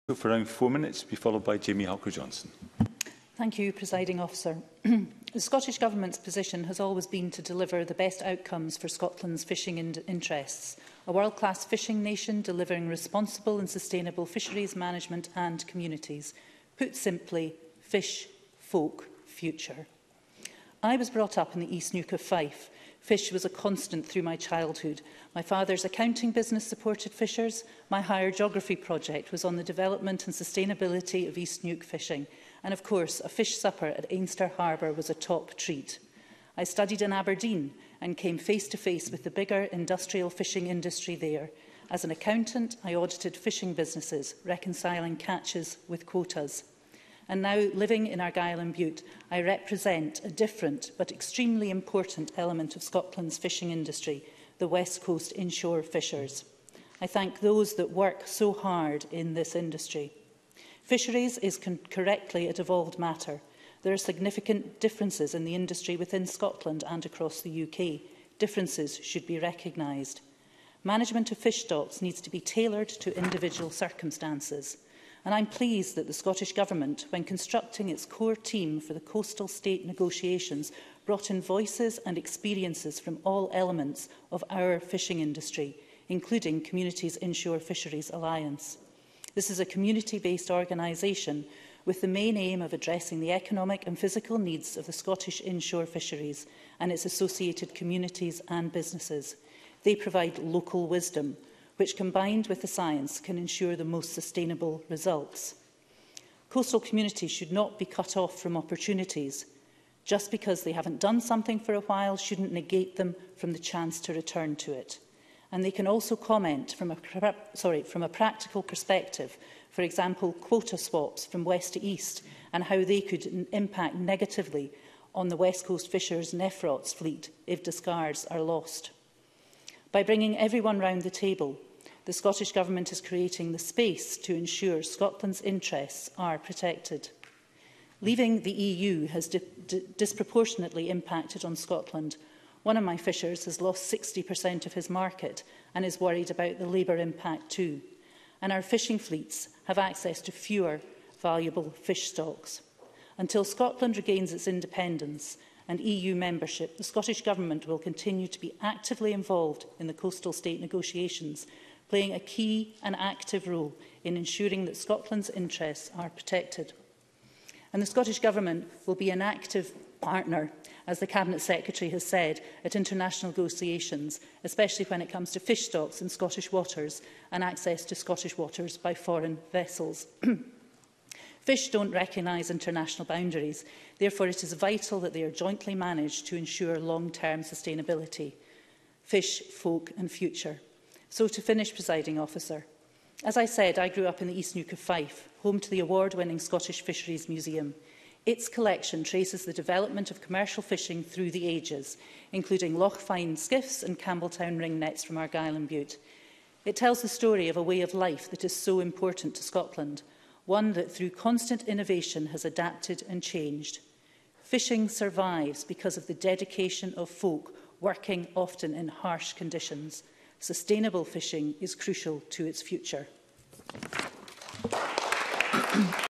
Contribution to Scottish Government Debate: Scotland’s Approach to 2021 Coastal States Negotiations
Jenni Minto MSP contribution to Scottish Government Debate: Scotland’s Approach to 2021 Coastal States Negotiations